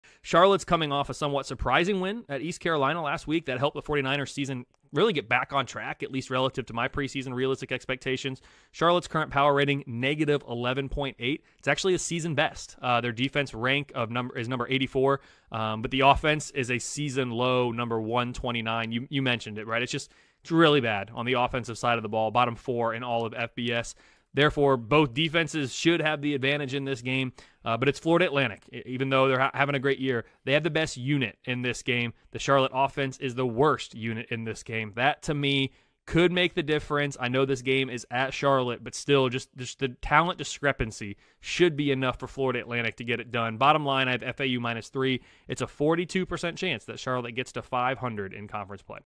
FLORIDA ATLANTIC (FAU) vs CHARLOTTE Predictions
Kelley Ford previews Florida Atlantic vs Charlotte in week 9 of College Football.